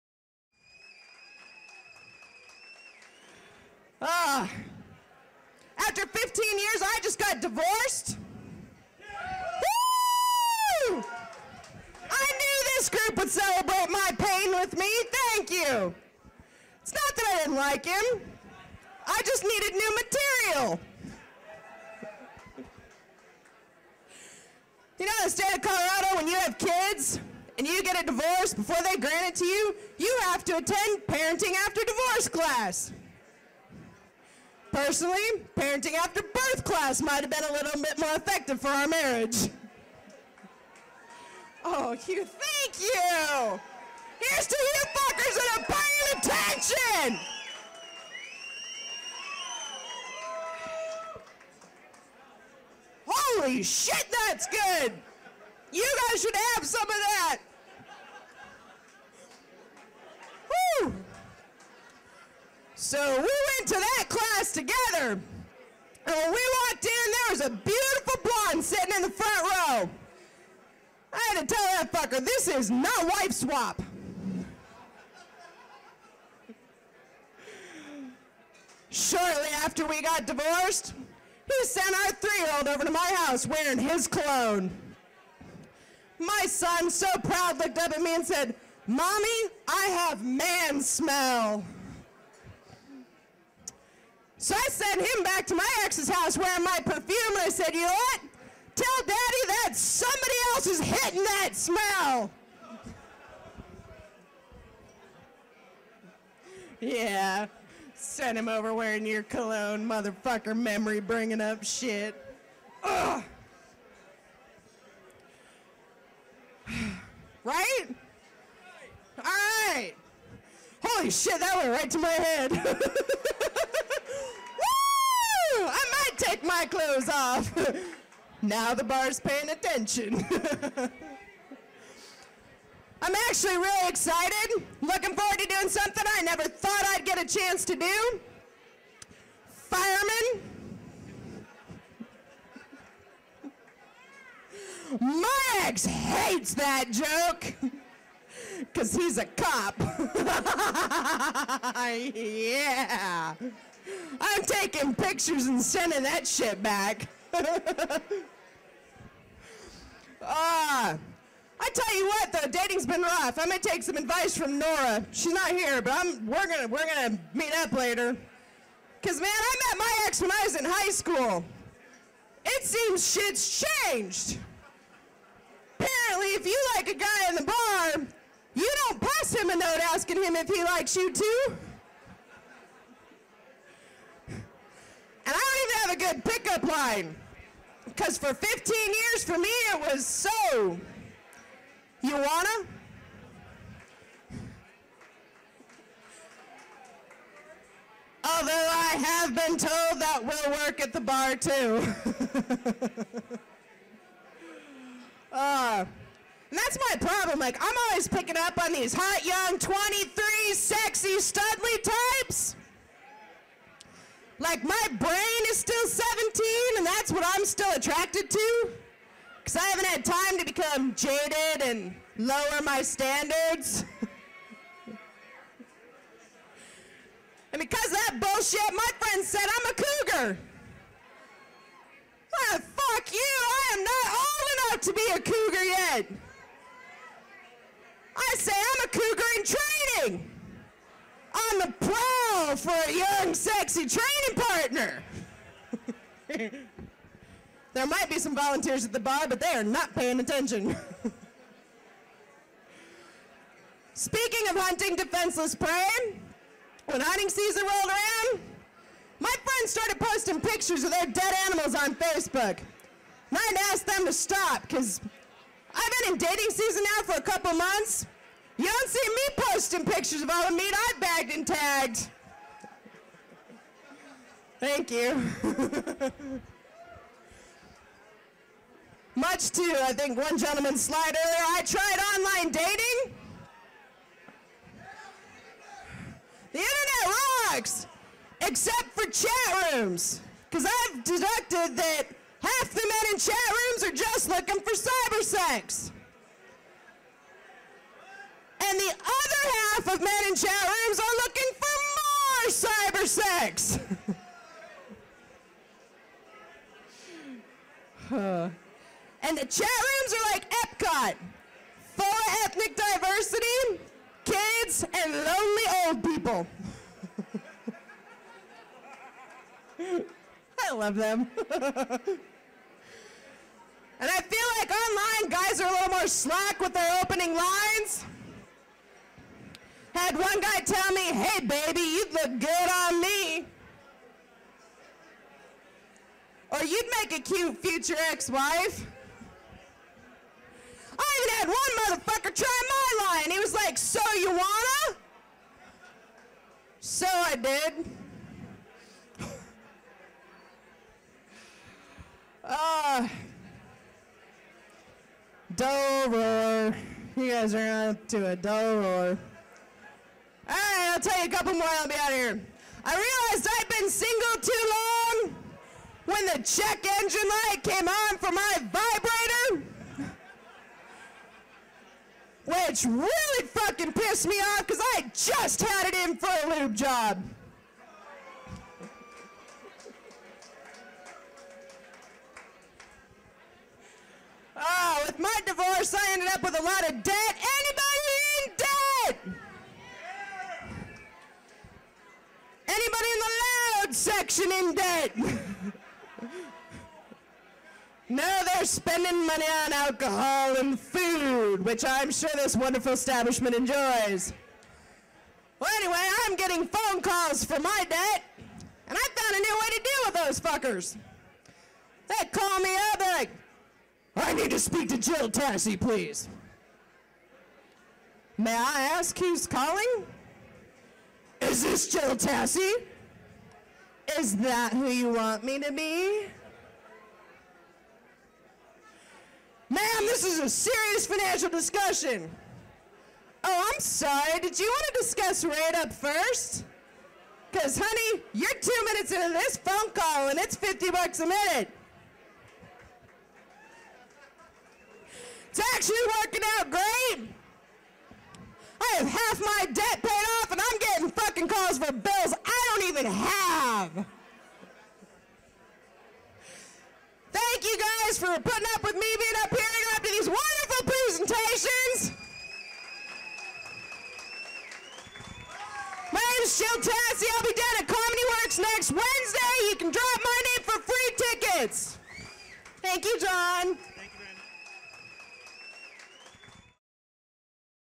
Ignite Denver 6: Intermission part 2 of 2
Jill Tassy Comedy during the Ignite Denver 6 Intermission - part 2 of 2